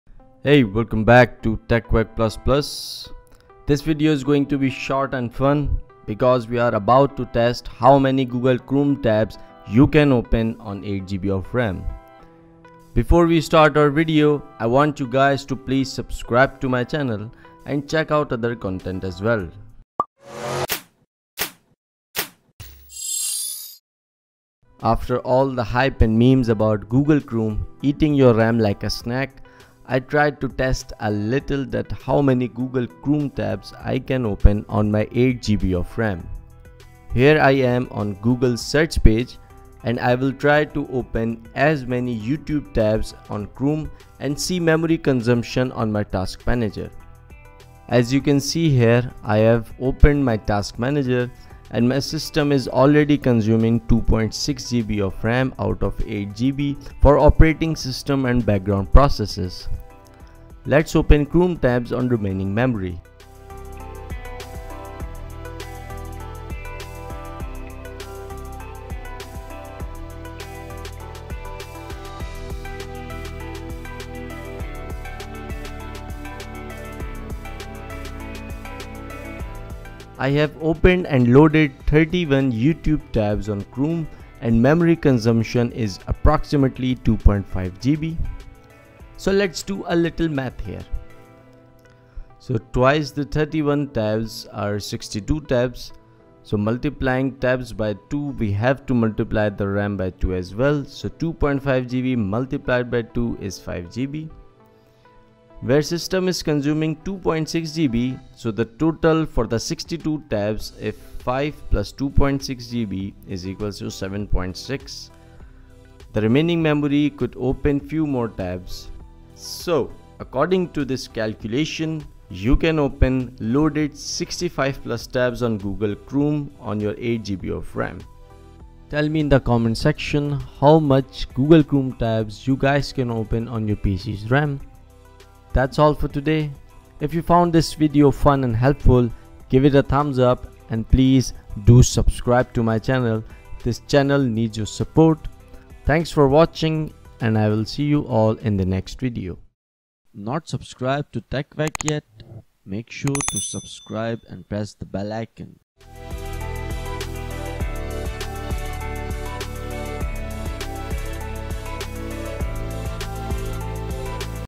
How many Google Chrome tabs 8 GB RAM can open ?
This video is short , fun and experimental .In this video I tried to open as many Google Chrome tabs on 8 GB RAM as possible. Did a little math with calculating the approximate results on how many Google Chrome tabs can you open with 8 GB. So, is your RAM worthy of being a Chrome RAM ?

Google Chrome has always been on the headlines for eating too much of the RAM. Memes of Google Chrome consuming too much RAM still circulates on the internet and still popular. The reason for this video is I wanted to find out if those memes are real , do Google Chrome really consumes too much RAM?So I recorded my experiment and shared it with you guys !

Watch the video to find out the answer !

Also , write in the comment section that how many Chrome Tabs you can open on your PC RAM.